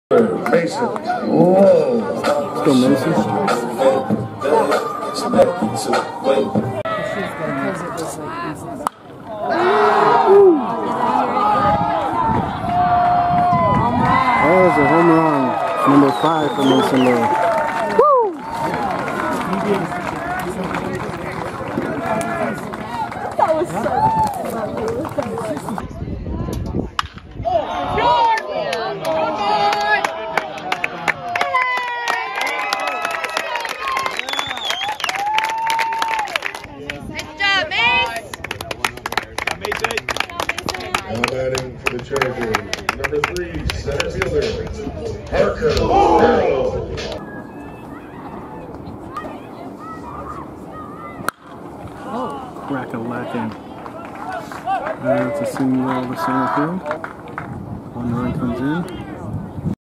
Chatfield's Mason Lowe Home Run, Single, 2RBI, 4 Runs | High School Baseball vs Bear Creek
Mason Lowe  hit his 5th home run 💣 of the season, a solo shot, had an RBI single, was hit by a pitch and scored 2 runs in the Chatfield Chargers16-1 win over Bear Creek 4.21.22.